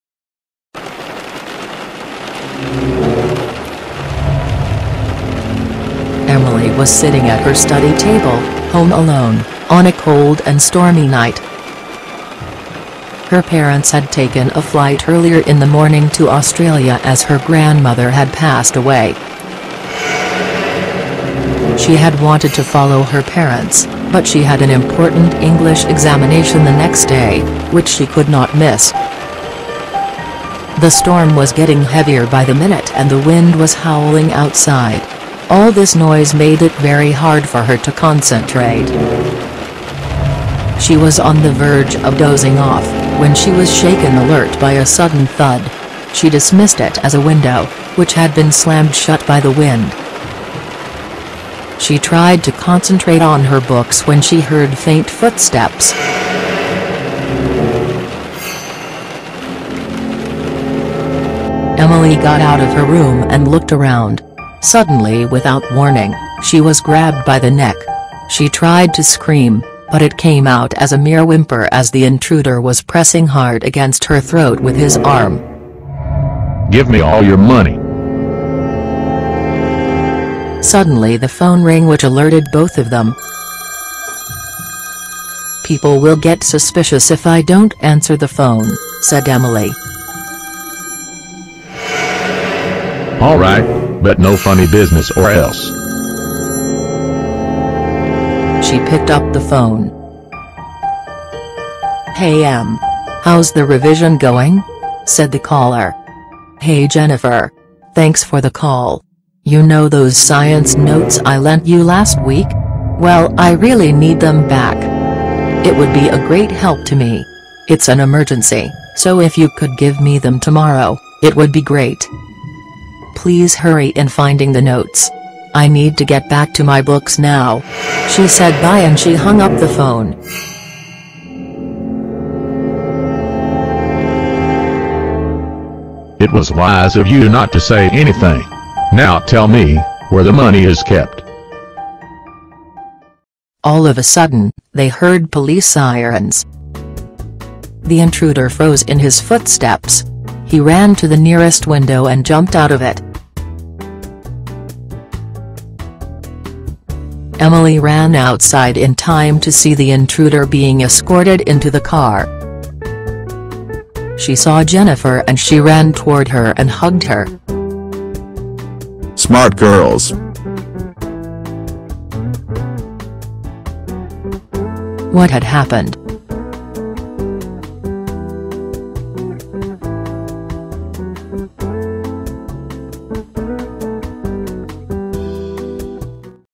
Riddles and Answers - #34 THE ROBBERY
Emily was sitting at her study table, home alone, on a cold and stormy night. Her parents had taken a flight earlier in the morning to Australia as her grandmother had passed away. She had wanted to follow her parents but she had an important English examination the next day which she could not miss... Try to solve this brain teaser. I hope you succeed.